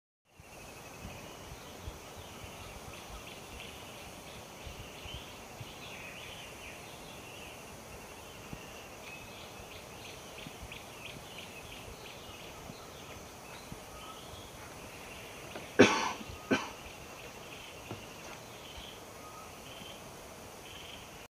Caterpillar eye mimicry.
This morning, I had a superb sighting of a caterpillar with a false eye. The detail was superb, even with highlights of reflected light spots in the false eye.